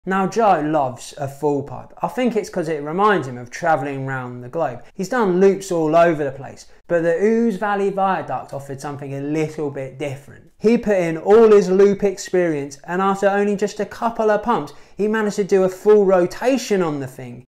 Only Joe Atkinson Skates Full Pipes like this!
Joe Atkinson is the best rollerblader for skating full pipes. He exceeded him self when he took his rollerblades to the Ouse Valley Viaduct in the UK and did a full loop.